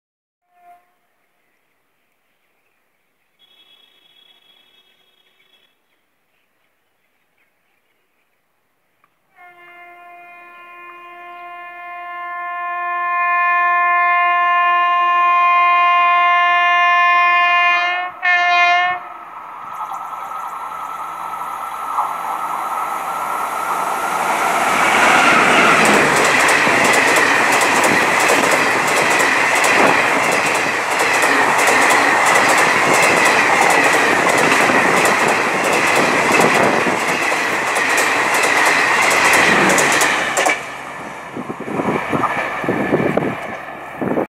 Vadodra Wap5 #30049 Powering 12216 Bandra To Delhi Sarai Rohilla Garib Rath Express Skips Kelve Road
Vadodra Wap5 #30049 in amul livery with the 4th fastest Train in Wr route(bdts-adi) Passes through kelva at Maximum speed.Because of the upgradation of line to 130 kmph,This train now gets regularly gets Wap5 till adi and from adi to dee alco(tkd wdm3a).Before that,It used to get e to e diesel.With Wr upgraded their line,Now there is rarely seen of diesel dominating :'( :(

Railfans can also be seen in action

Location:-Kelva road

Date:-29/12/15